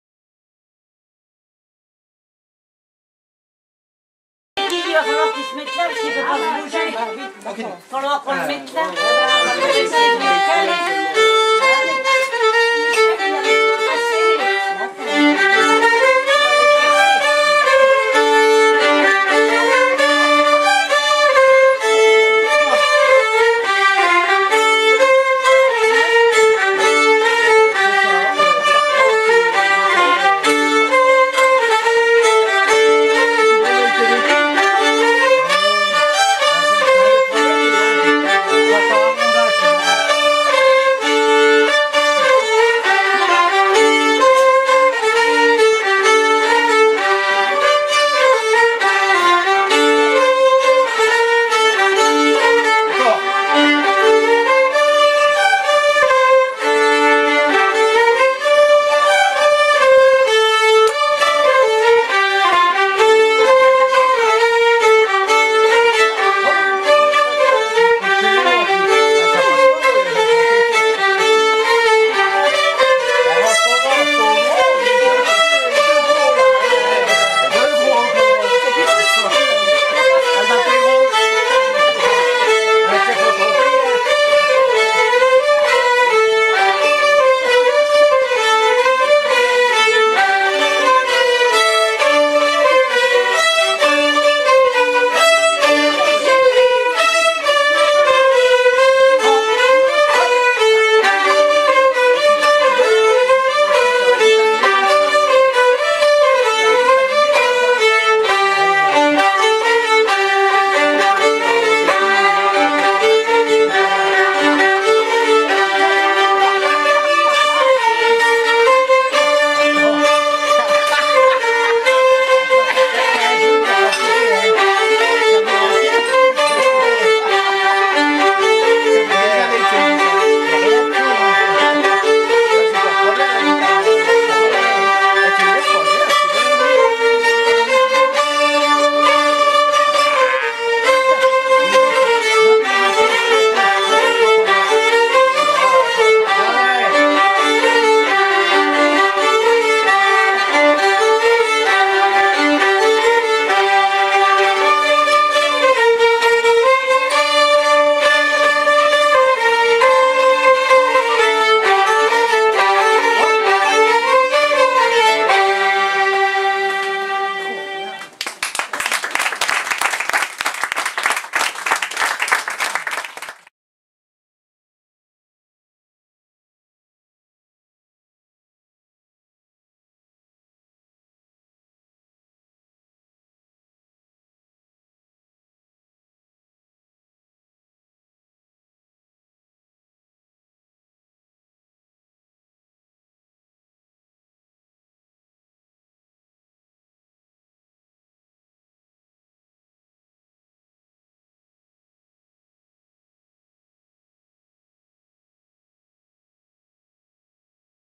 Les violons de rézonances à Luzy
Le groupe des violons de rézonances s'est produit dans le cadre de la 15 ème édition du festival du violon de Luzy au bar PMU de la ville;